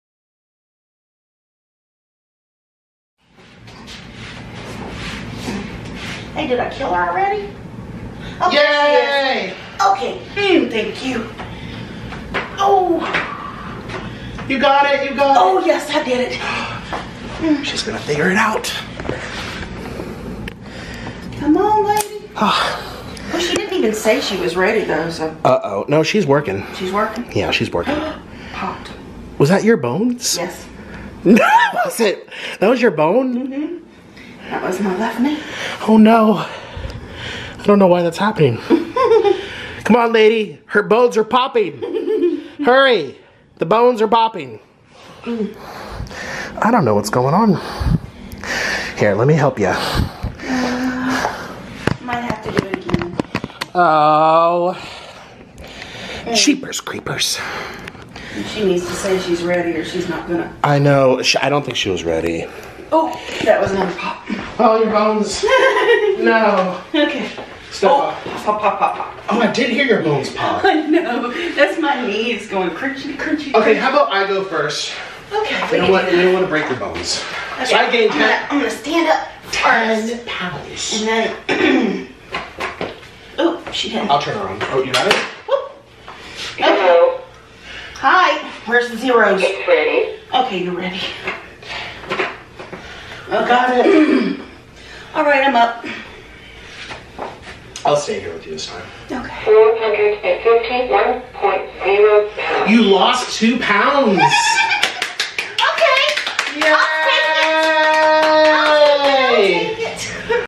Hungry Fatchick’s Knees Snapping While Standing
While trying to weigh herself, Candy’s knees snap due to her massive size.